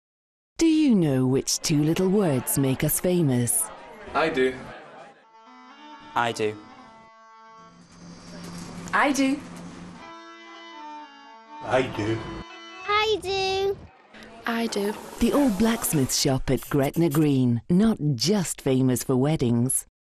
Gretna Green Old Blacksmiths Centre
The Old Blacksmiths Centre at Gretna Green is the perfect venue for weddings, shopping, tartan, food and a relaxing coffee.